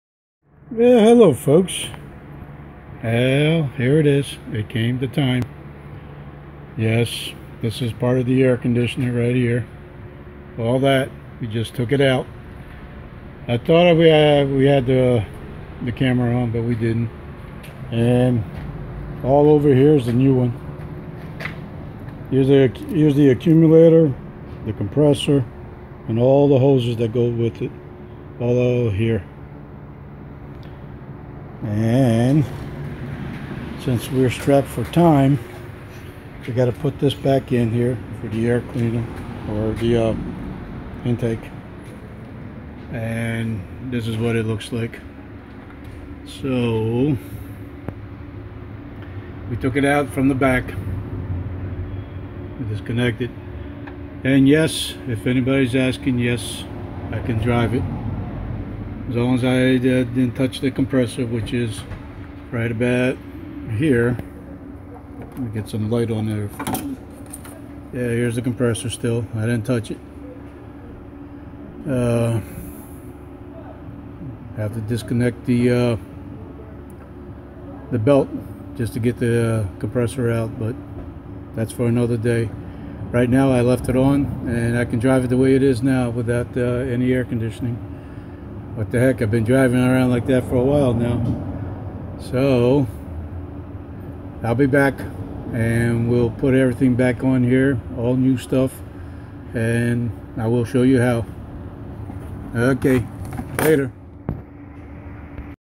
F 150 air Condition 1990
Repairing my air conditioning system on my 27 year old F 150
With more to come